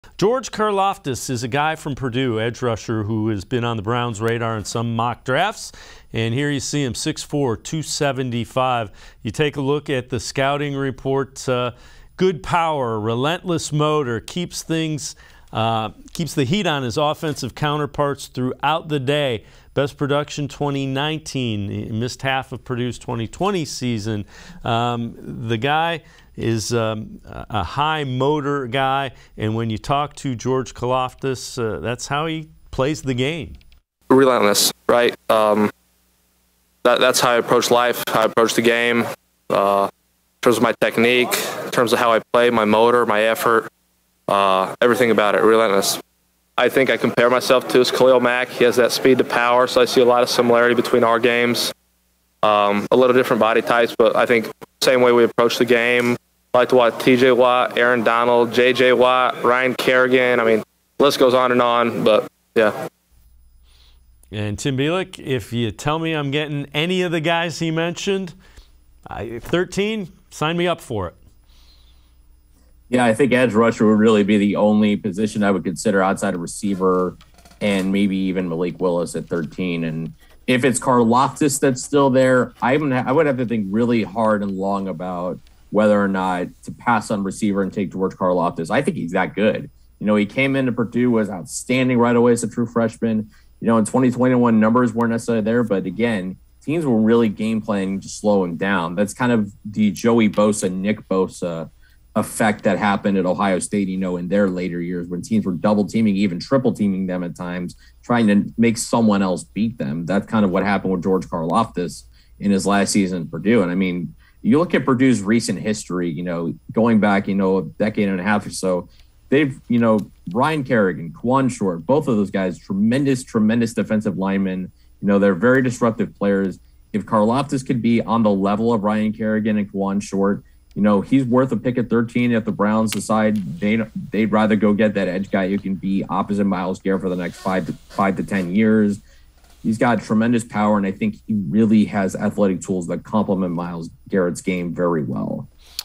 Scouting Browns Draft Prospect George Karlaftis - Sports4CLE, 3/9/22
Dave Bacon and Tim Bielik discuss Purdue defensive lineman George Karlaftis.